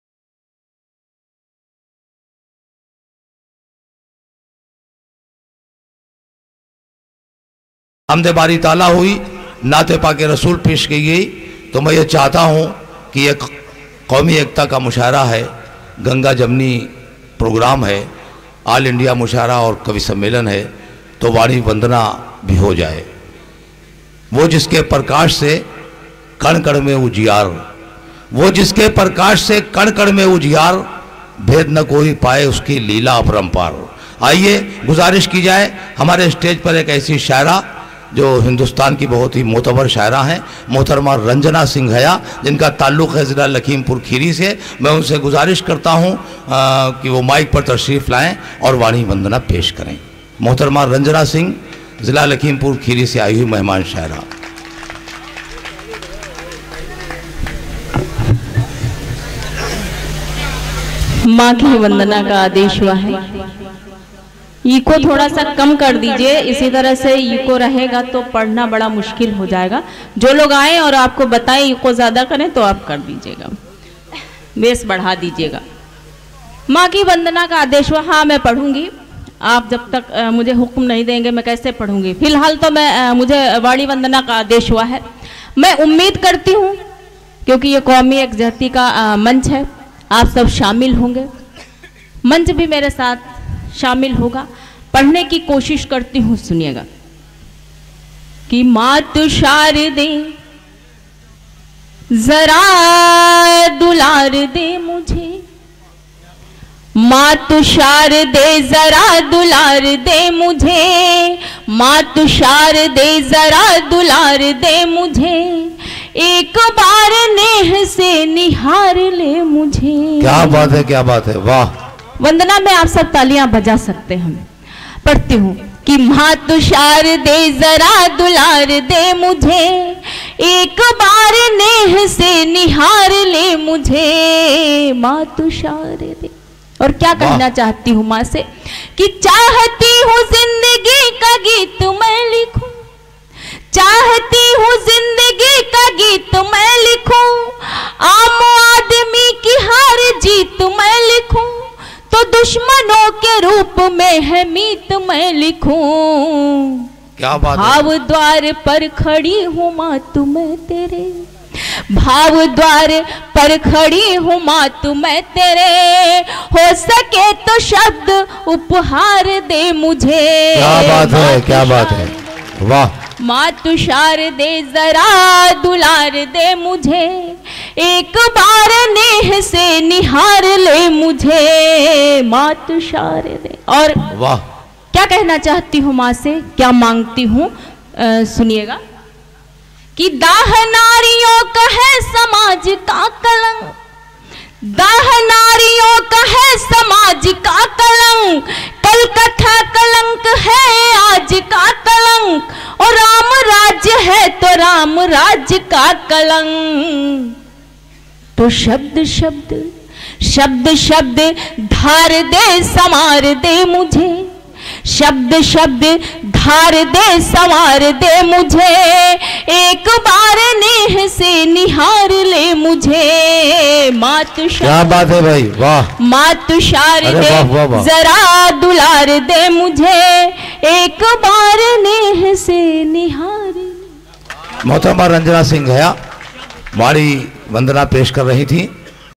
RANJANA SINGH HAYA,ALL INDIA MUSHAIRA,RAI BARI MAHUAWA,WEST CHAMPARAN,07TH DEC 2017.
RANJANA SINGH HAYA,
EK SHAM KAUMI EKTA KE NAAM,
ALL INDIA MUSHAIRA HELD ATRAI BARI MAHUAWA,WEST CHAMPARAN,BIHAR.
ON 07TH DEC 2017.
PRESIDENT-NAUSHAD AKHTAR